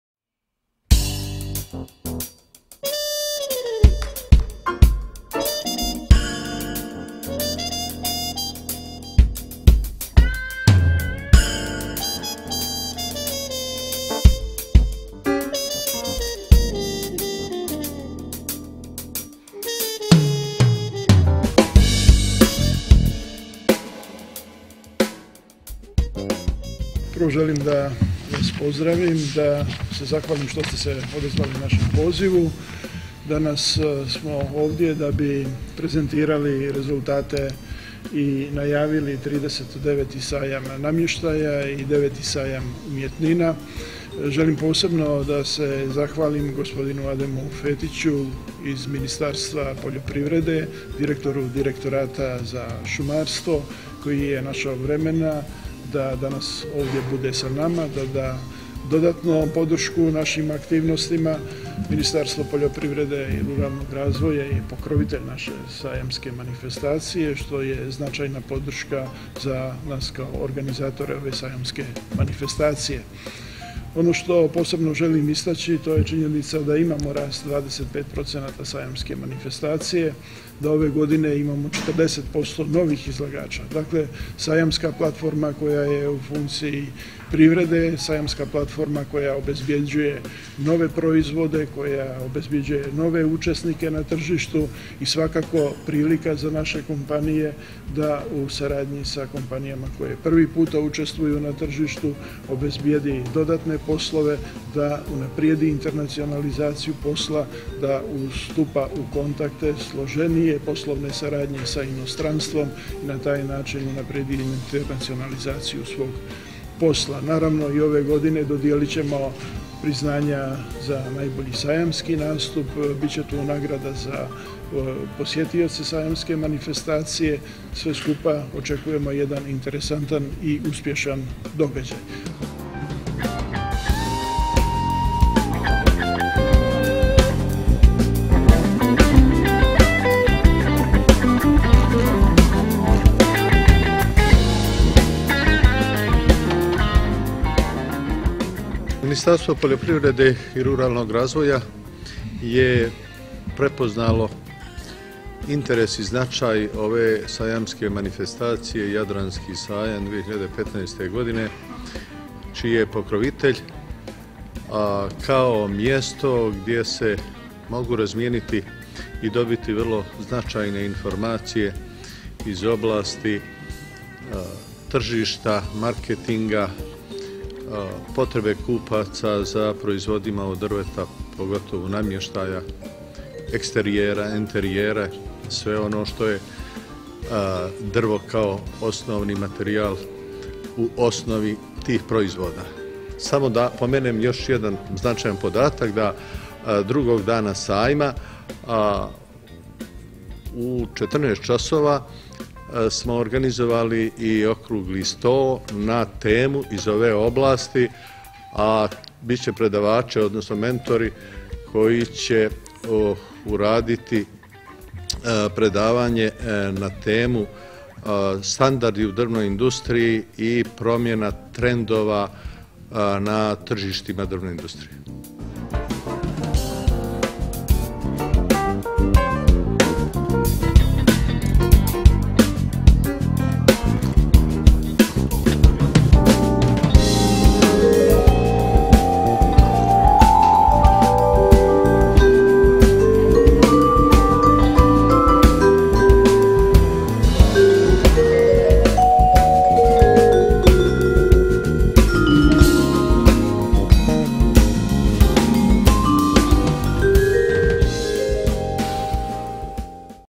39. Sajam namještaja i 9. Sajam umjetnina - Press konferencija 24.03.2015. Jadranski Sajam Budva
39. Sajam namještaja i 9. Sajam umjetnina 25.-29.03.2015. Jadranski Sajam Budva